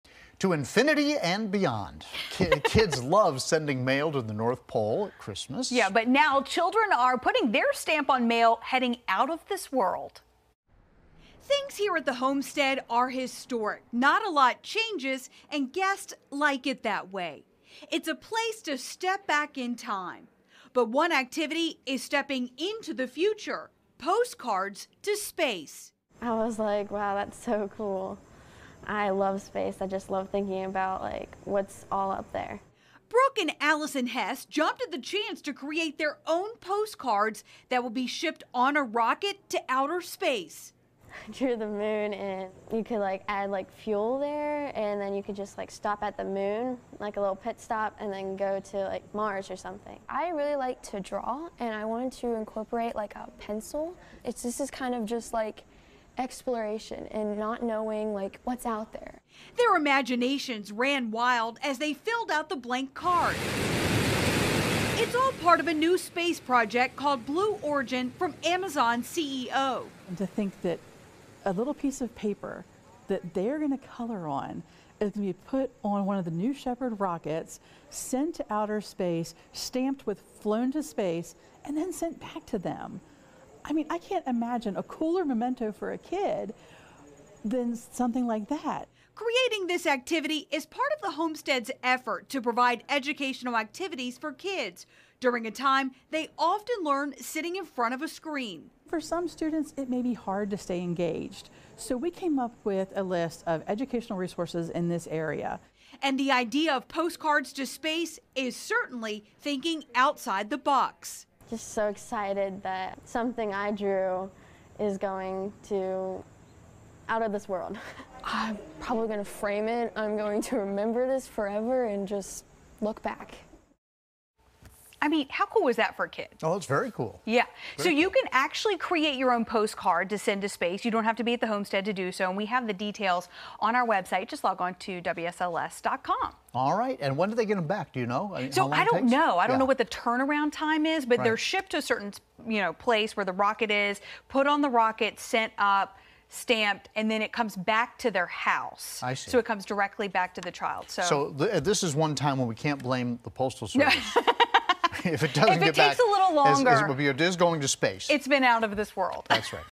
Local students send postcards into space
Kids can now send mail to infinity and beyond.